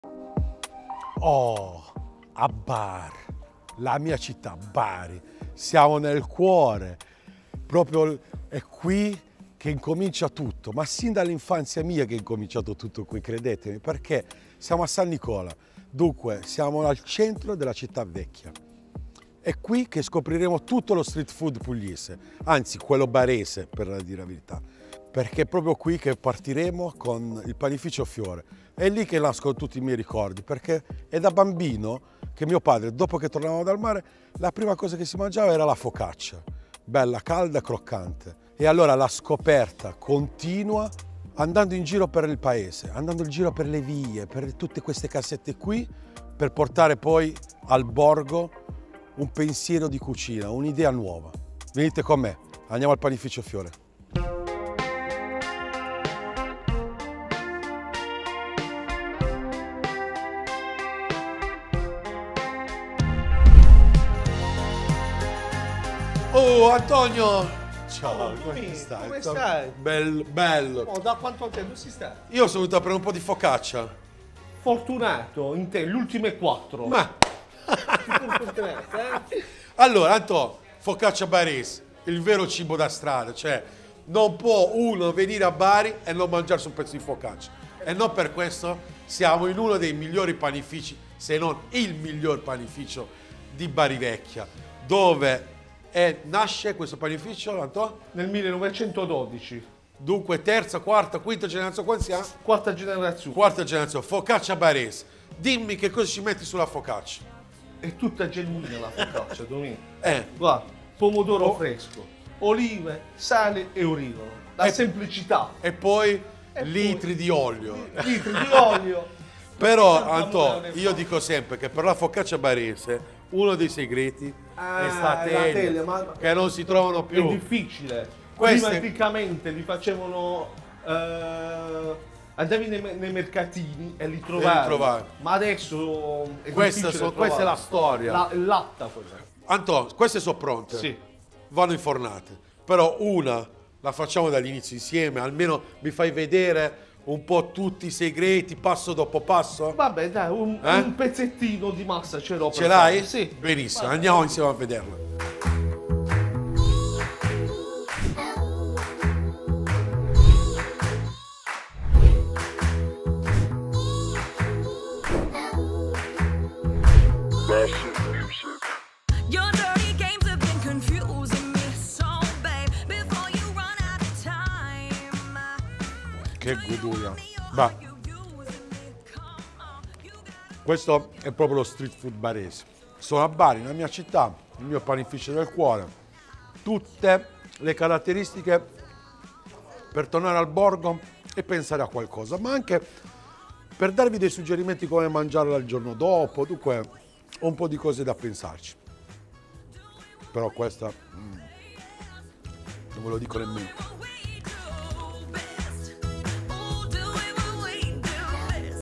LA VERA FOCACCIA BARESE - Puglia Food Stories
Lo chef Domingo Schingaro ci accompagna alla scoperta dello street food pugliese